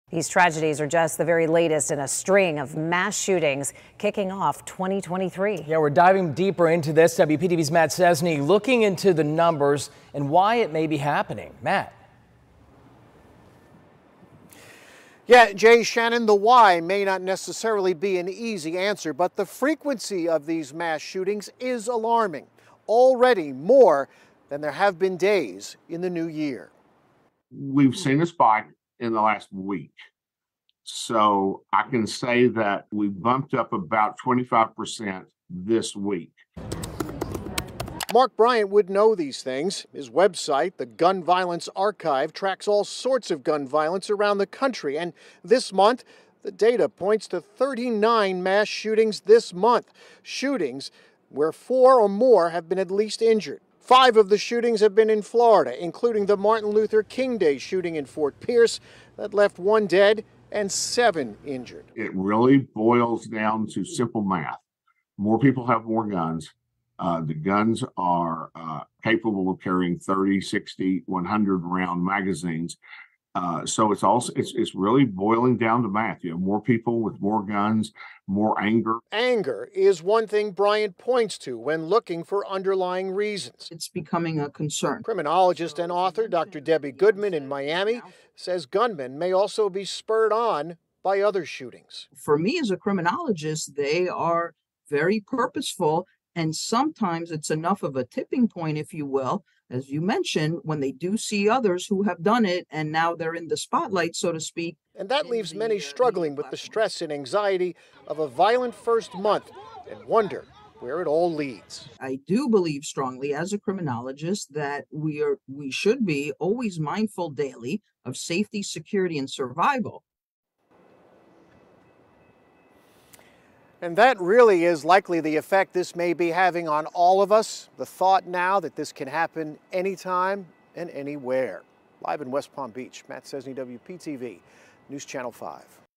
U.S. sees spike in mass shootings to begin 2023
It's been a violent start to the new year in the United States with more mass shootings than days that have occurred so far this year.